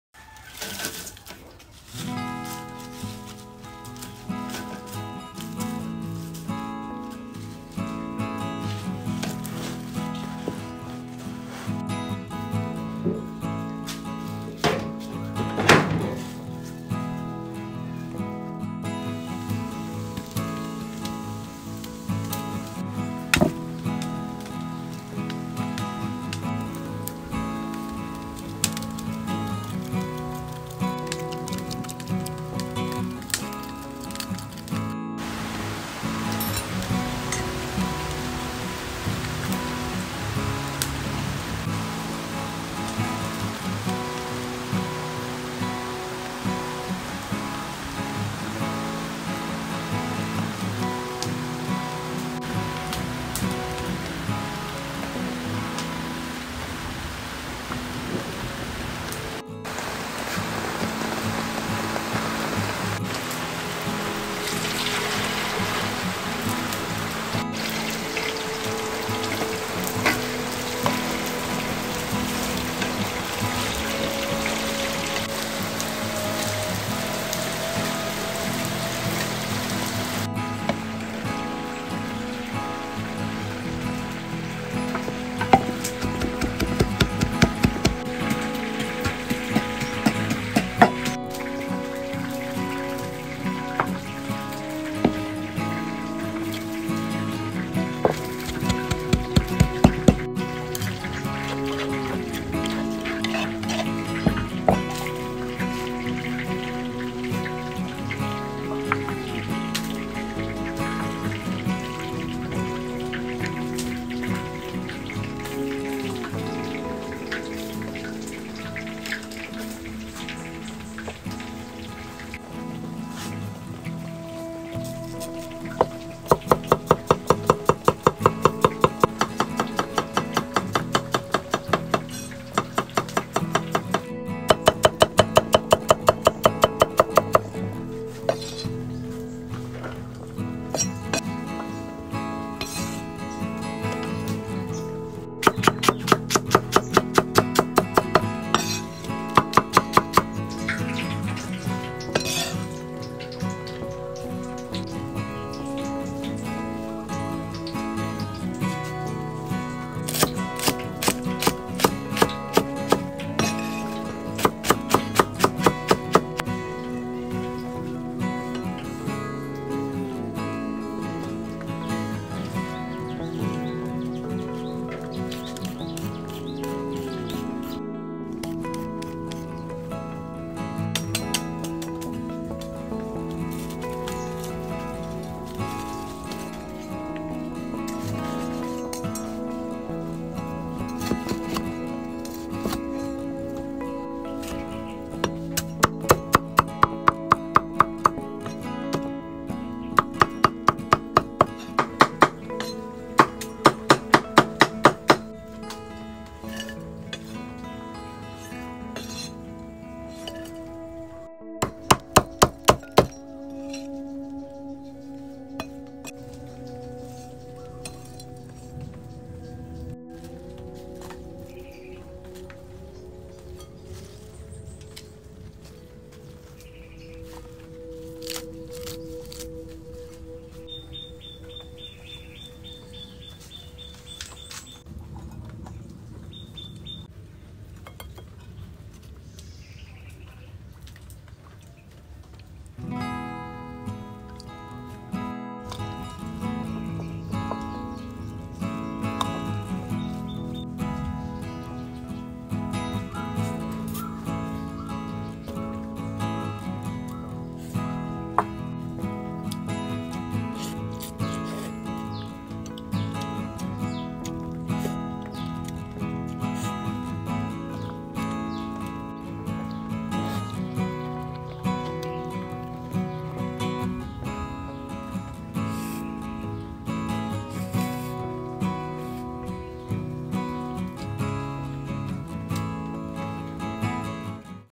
Delicious big pork leg cooking by pregnant chef - Cooking with Sreypov
Hello friends. Let's enjoy with my new video.